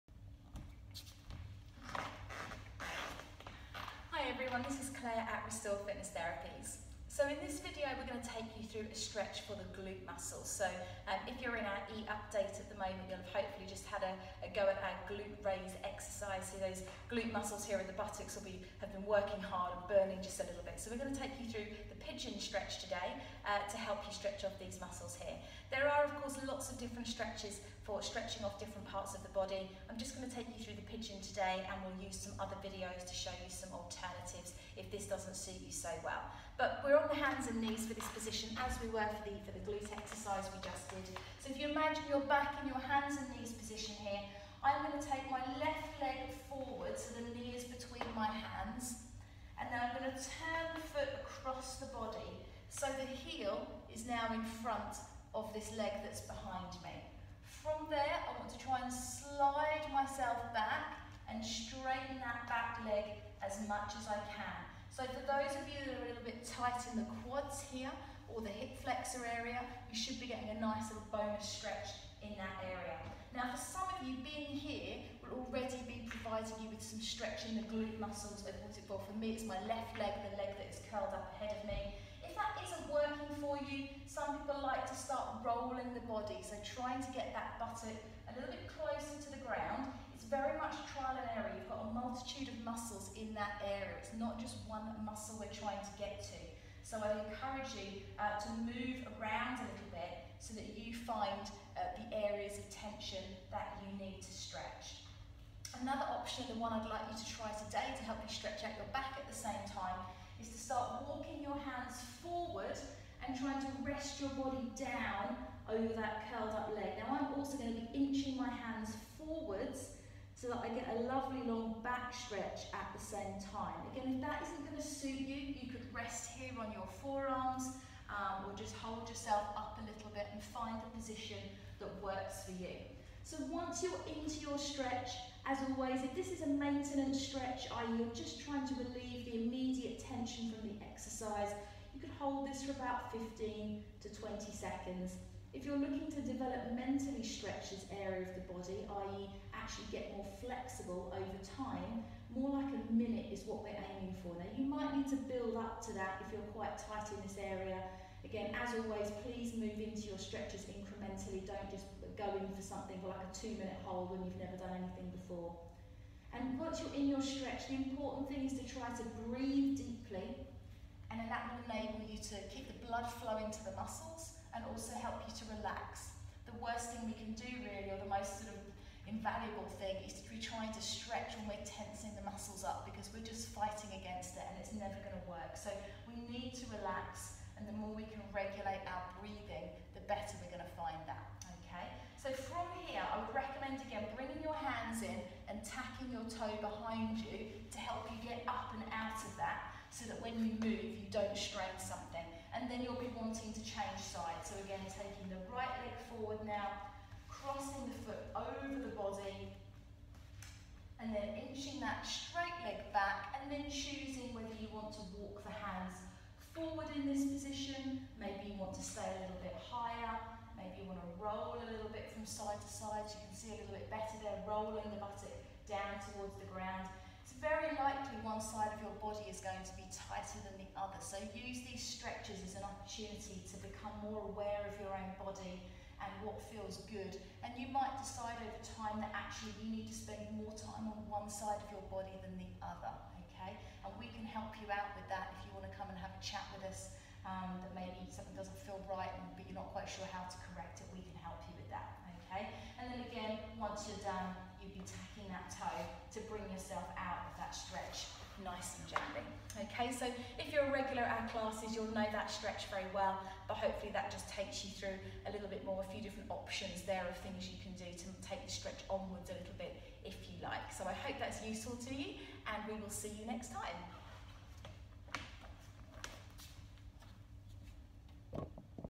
Pigeon Stretch (for Glutes)
My favourite stretch for the Glute muscles (buttocks)